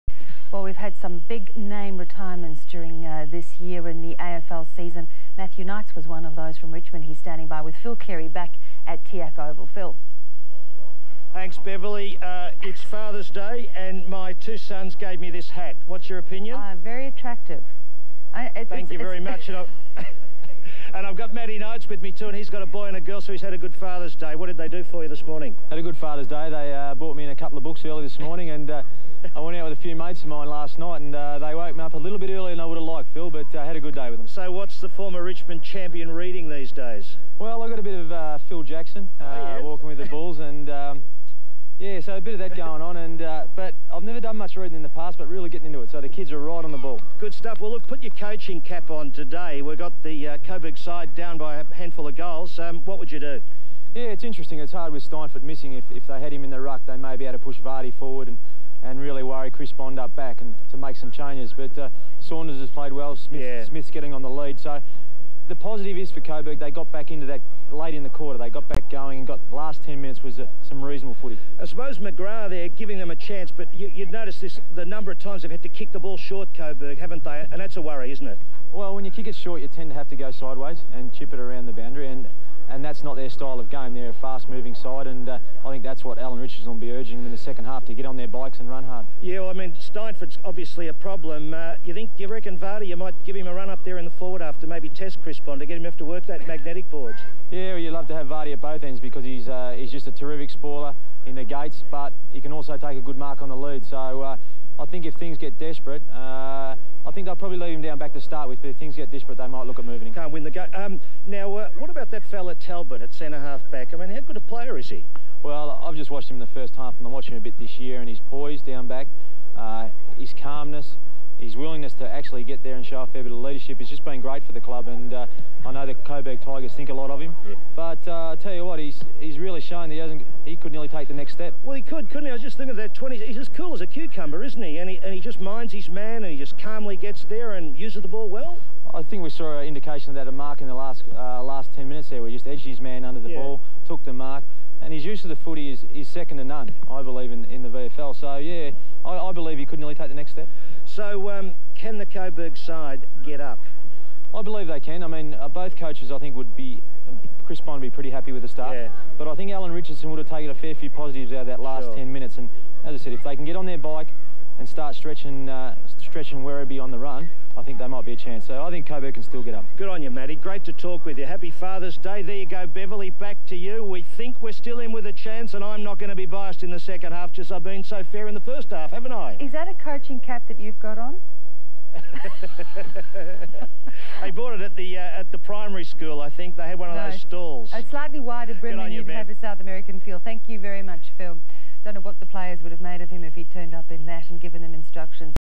ABC Sport Matthew Knights Fathers Day Interview With Show Off Kid In Background 2002
Found on a tape copy of 'Werribee Vs Coburg VFL'

Head over to Batprince's game channel:
Youtube.com/Batprince
OR
Head over to Pete's Twitch
Twitch.tv/Peterdea